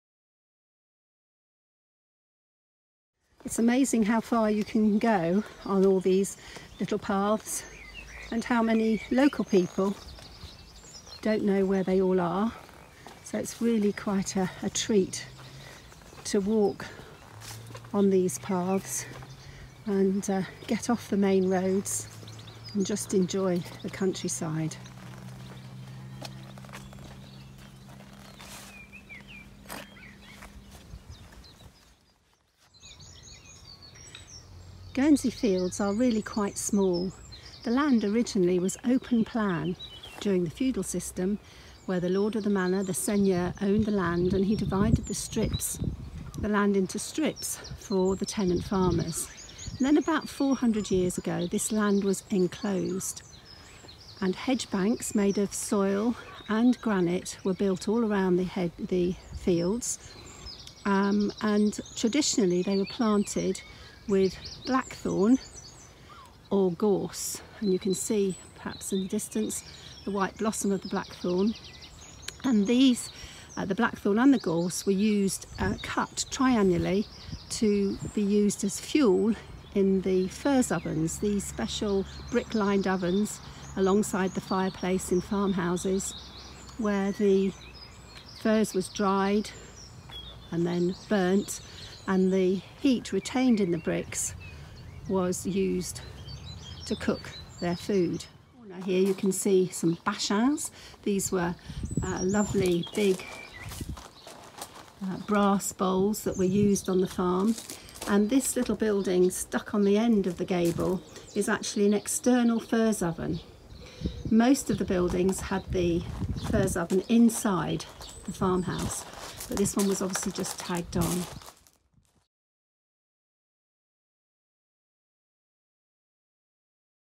Guernsey Countryside - Virtual Walking Festival Taster
With Gold Accredited Guide – Gill Girard